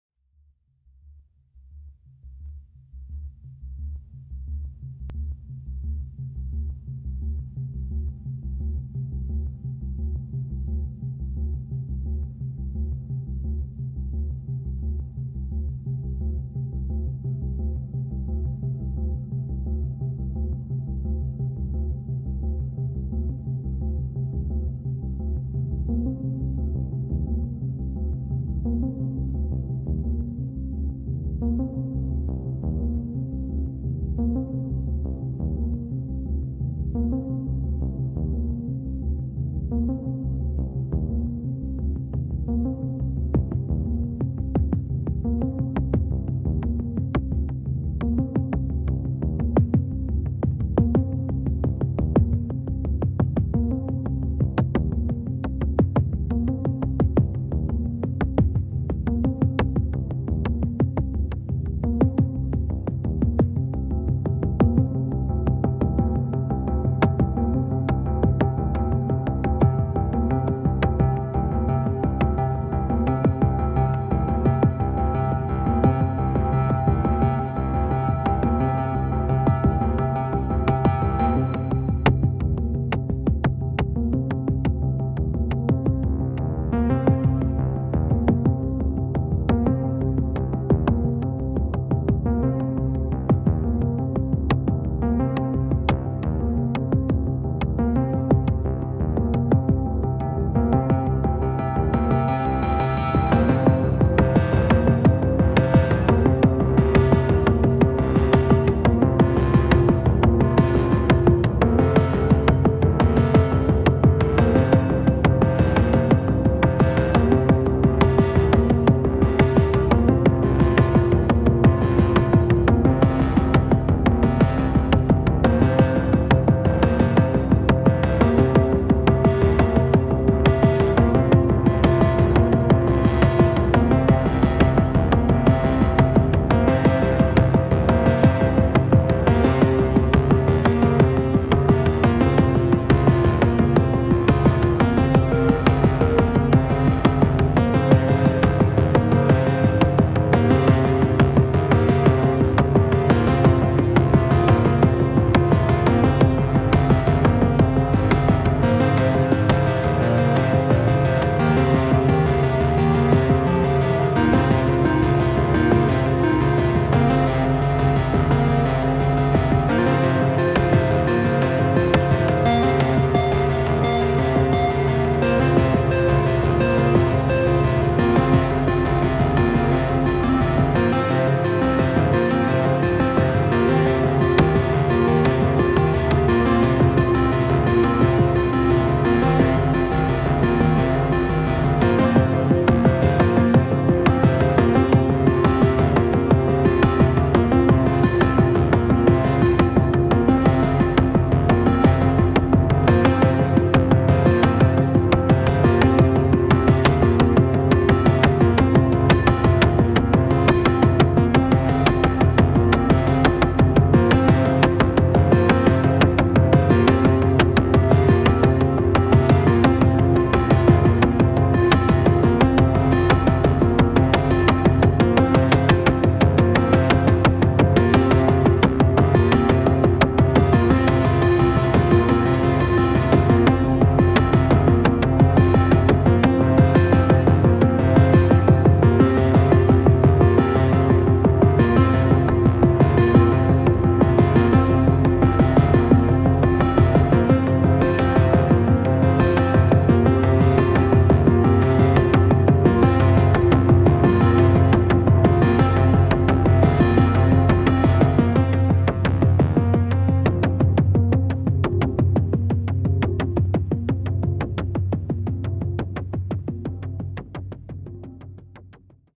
Jamuary2021 - Day 08 // Subharmonicon, DFAM, Crave
Improvisation with the Subharmonicon, DFAM and Crave. Played with patching the pitch of the DFAM through to the Subharmonicon trigger as well as back into the DFAM noise so I can have triggers on a VCO even when the sequencer isn't moving for a steady pulse.

NOTE: Apologies about the left-panned audio, I realized too late after posting and it was due to a bad cable connected to the recorder.

Jamuary2021 - Day 08